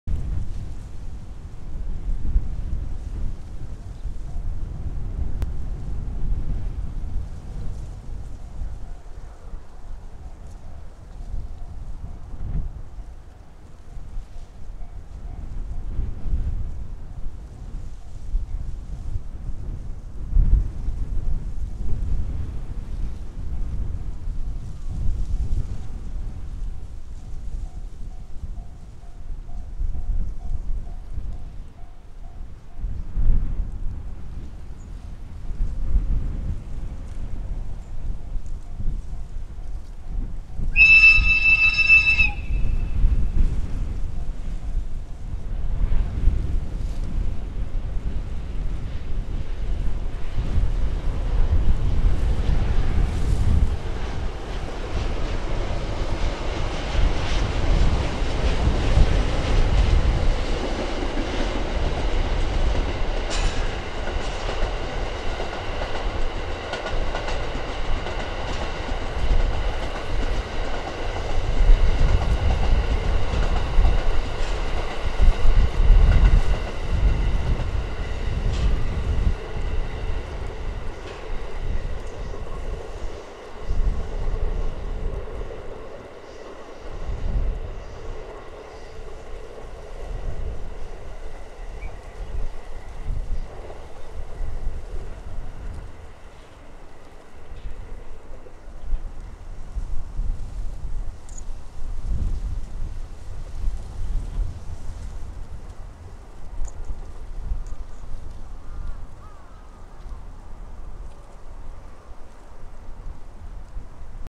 【残り3週間】寝台特急カシオペア2016.2.28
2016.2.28 東鷲宮〜栗橋にて撮影。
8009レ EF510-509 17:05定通